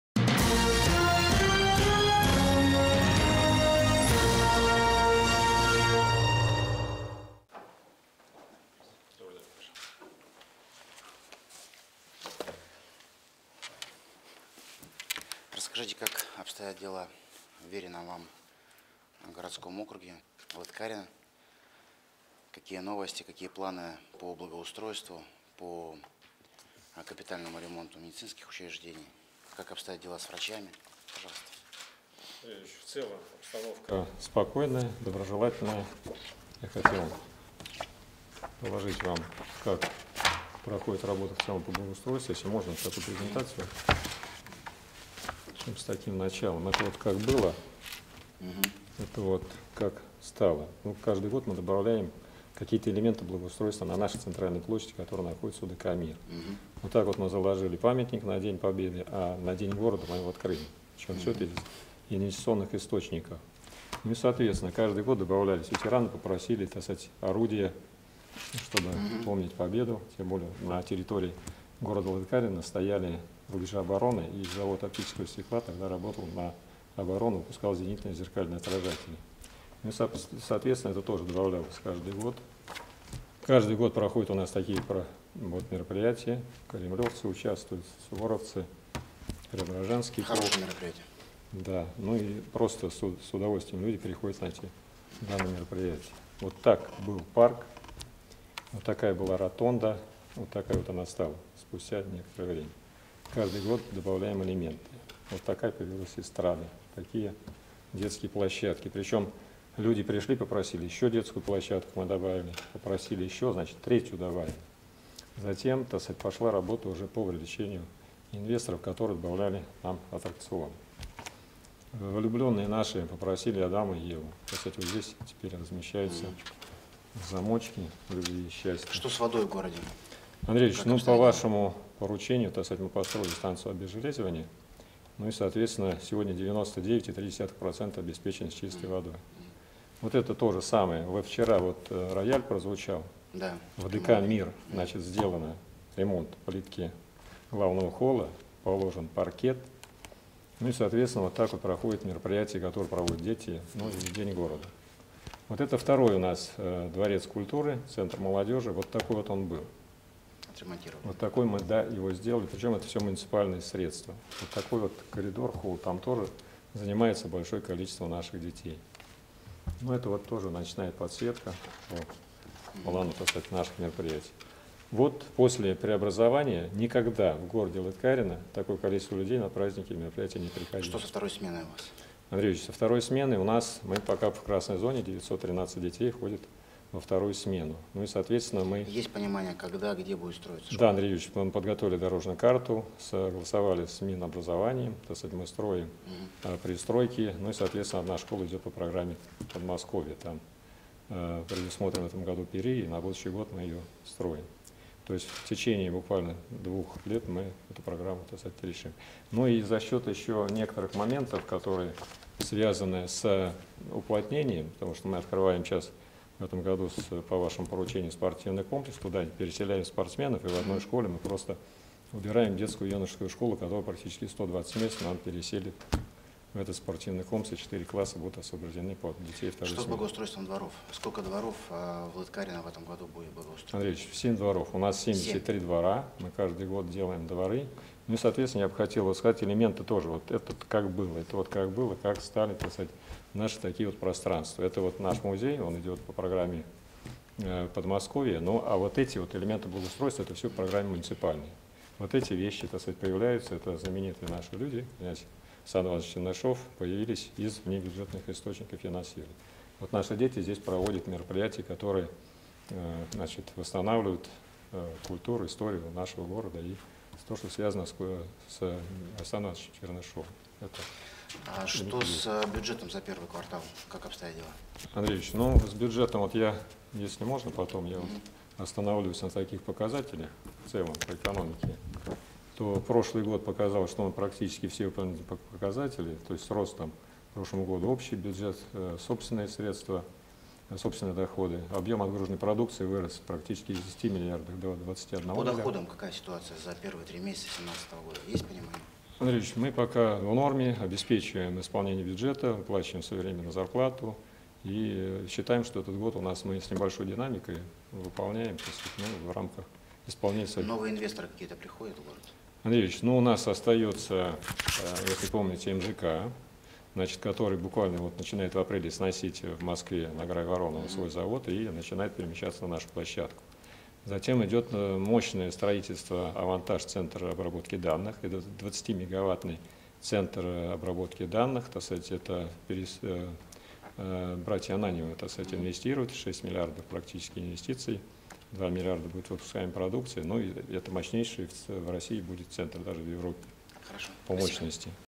Почти 100 процентов лыткаринцев обеспечены чистой водой
Обеспеченность чистой водой в подмосковном Лыткарине составляет 99,3 процента. Об этом на встрече с губернатором Андреем Воробьевым сообщил глава муниципалитета Евгений Серегин, передает корреспондент «Подмосковье сегодня».